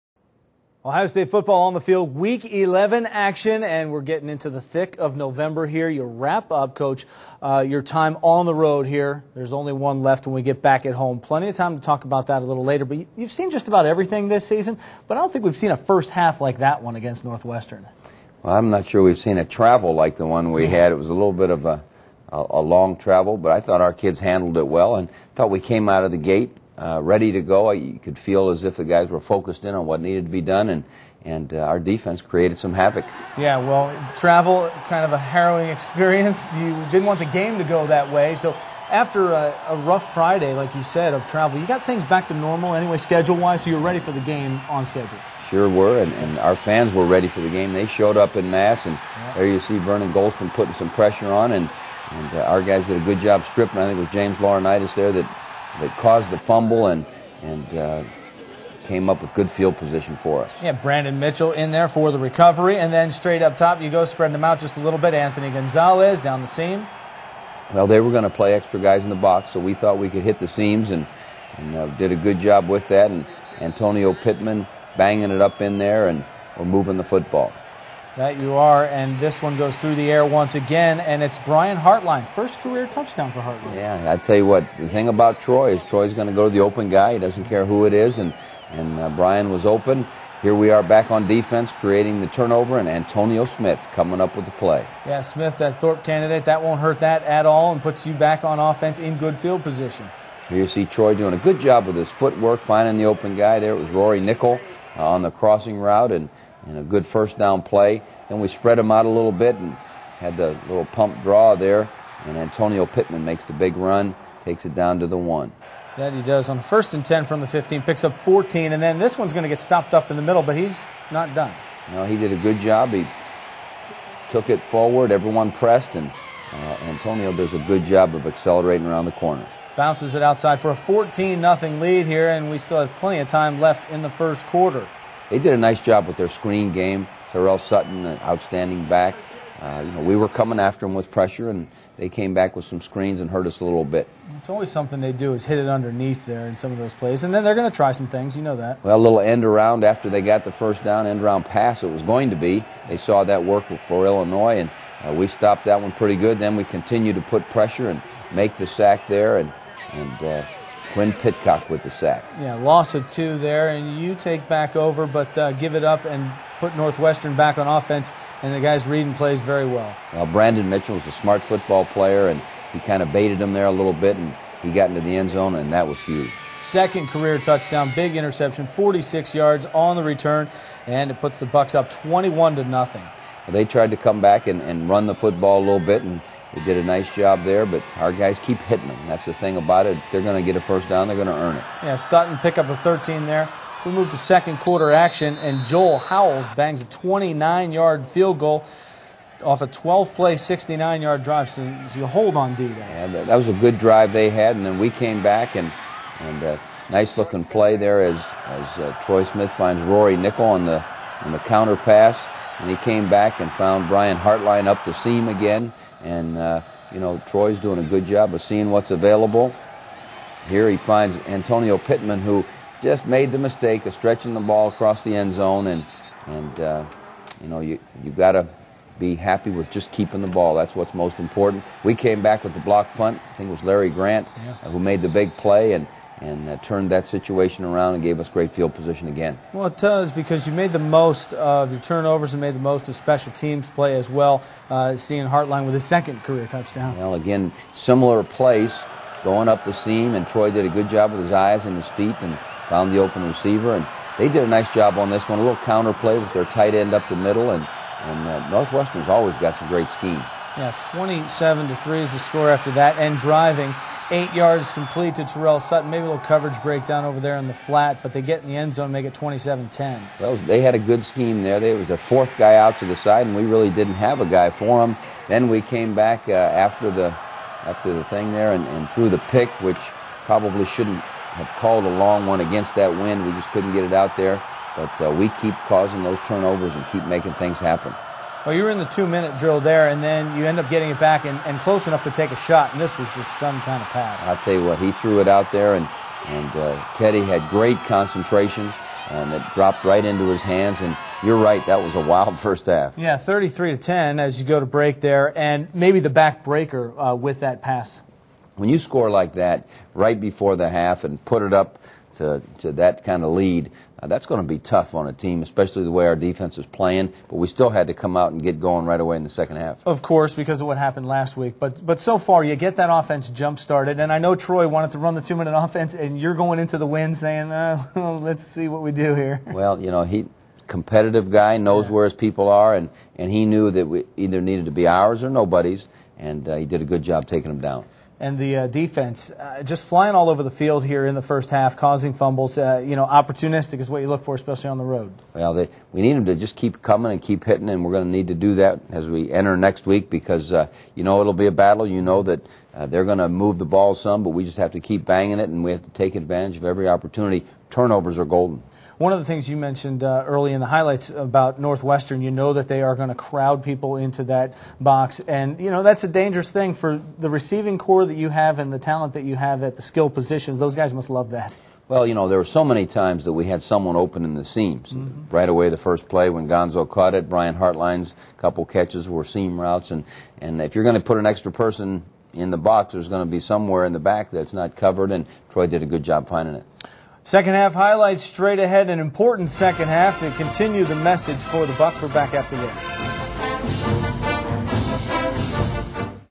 Ohio State vs Northwestern, 2006
Highlights of the Buckeyes 54-10 victory over the Wildcats